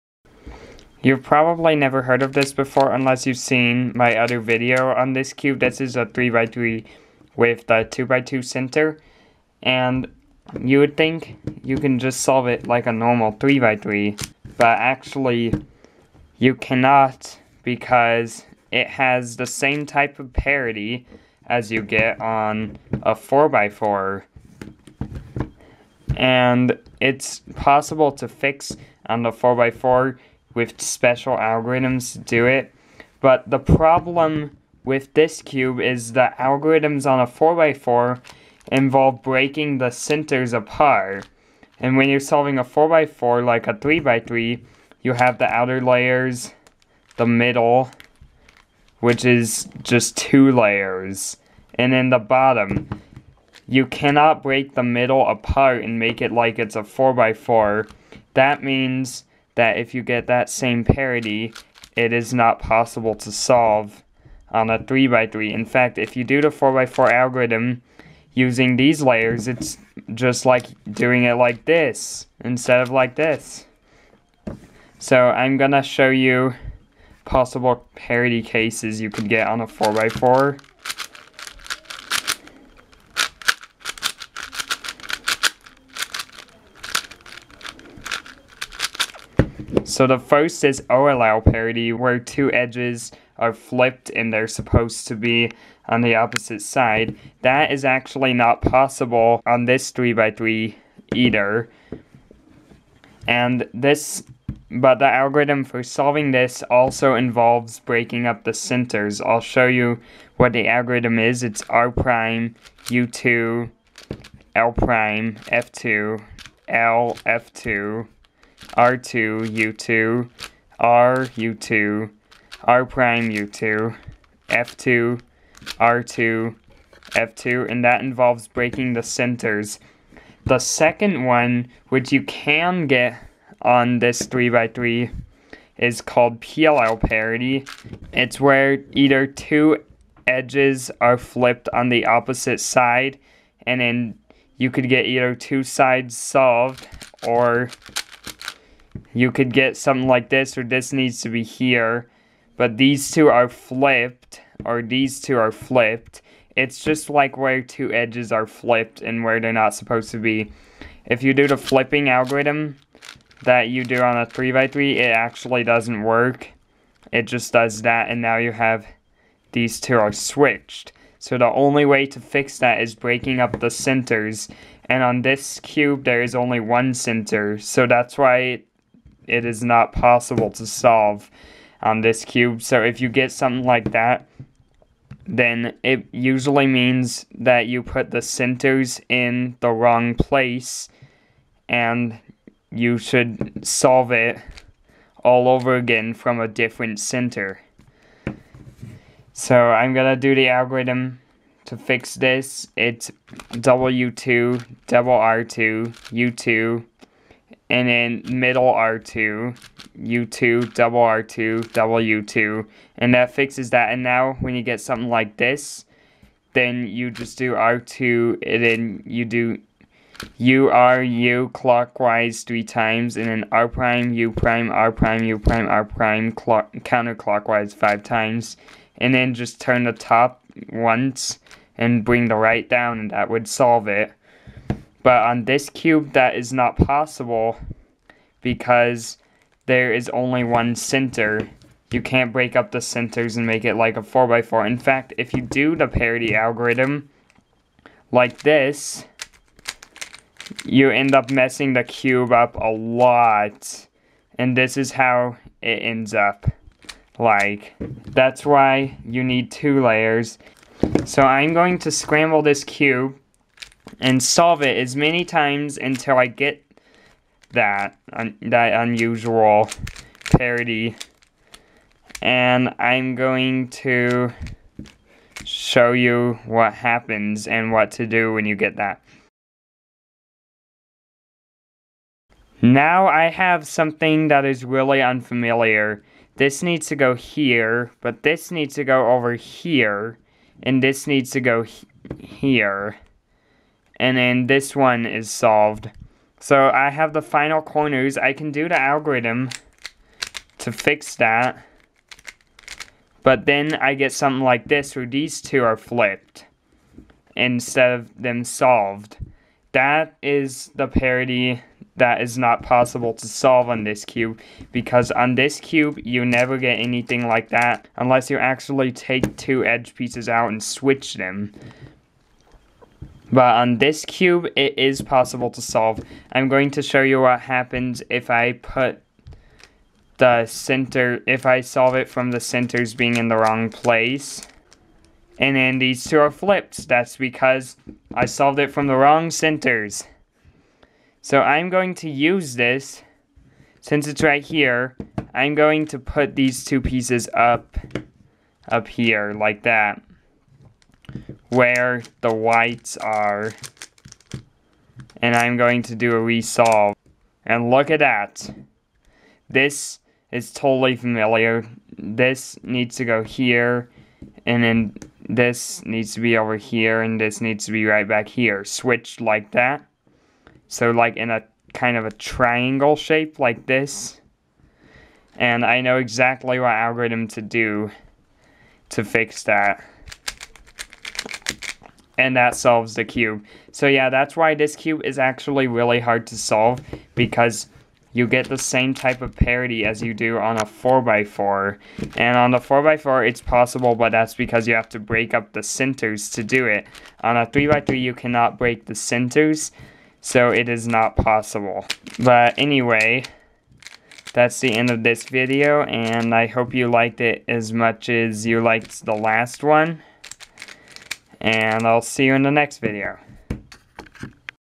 Why is PLL Parity Impossible on a 3x3 cube?
This video shows when solving a 3x3 you don't get PLL parity like on a 4x4,
You use a special algorithm for fixing it on a 4x4 but on a 3x3 it is impossible.

0:00 Same parity as a 4x4
1:17 Parity errors on a 4x4
4:04 Parity algorithm on a 3x3
4:27 Solving
6:42 Outro